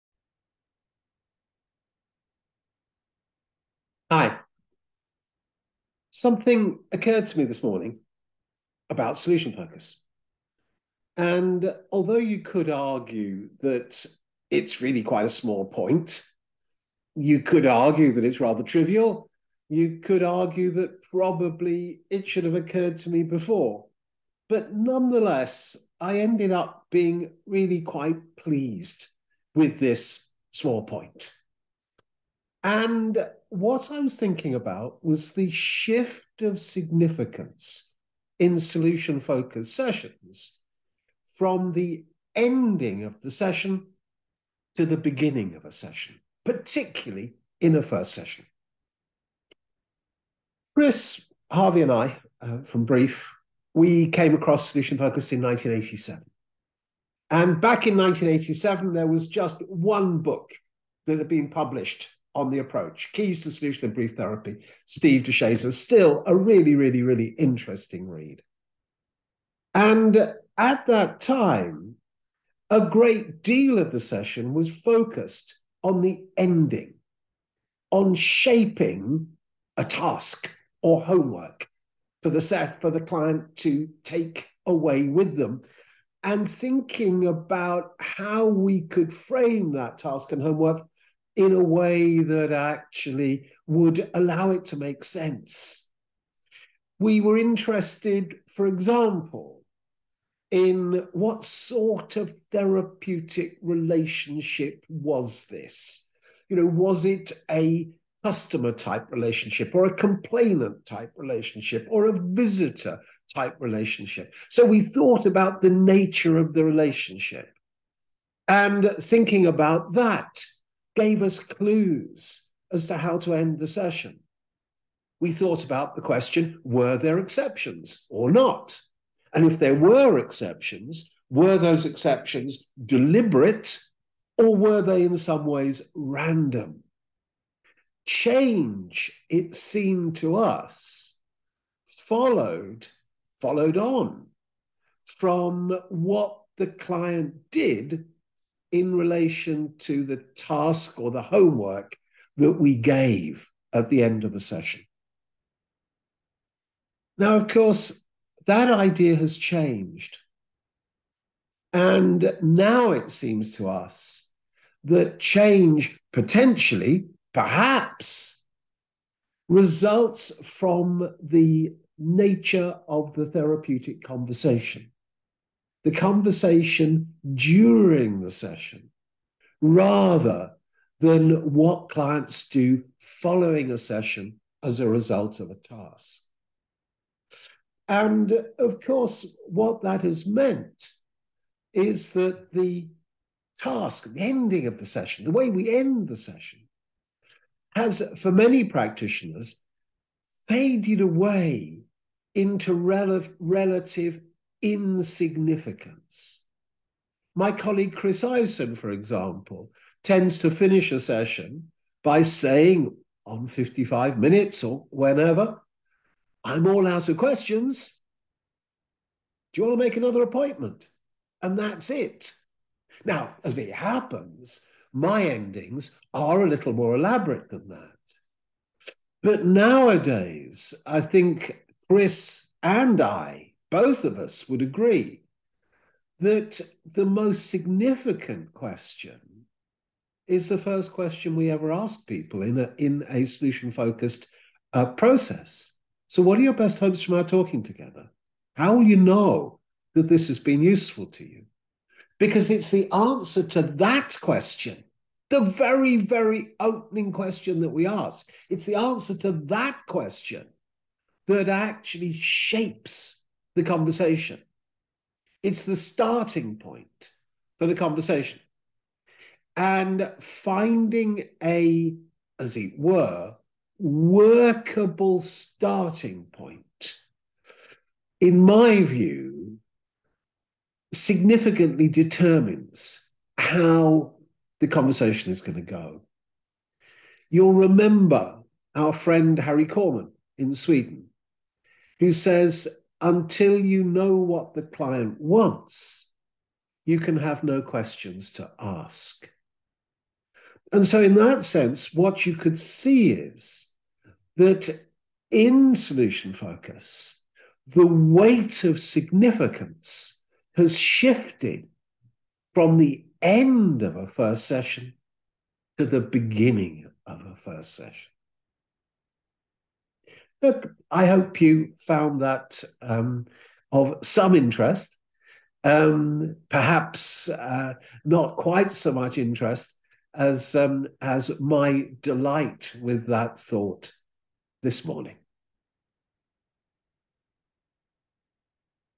Endings to beginnings
The shift in significance from endings to beginnings in first Solution Focused sessions tells us about how the approach has developed and indeed changed.